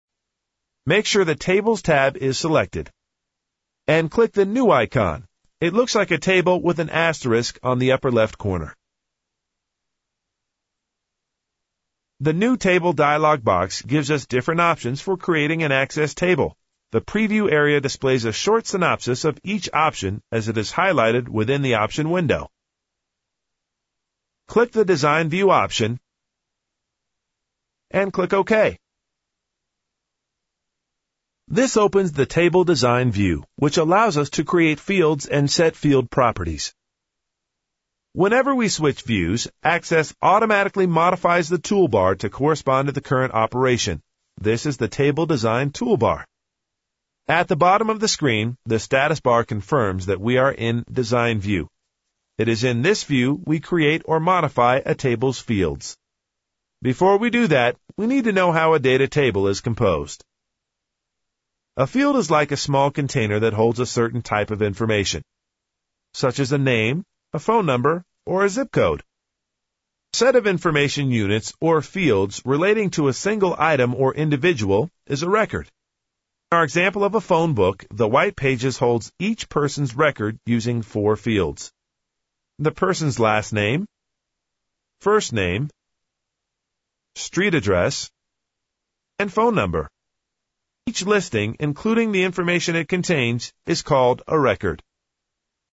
How to Create a Table in Microsoft® Access®
This Video contains instructions on how to create a table in Microsoft®  Access®. This powerful video comes from Video Professor Where we have educated over 10,000,000 computer users on Microsoft®  programs.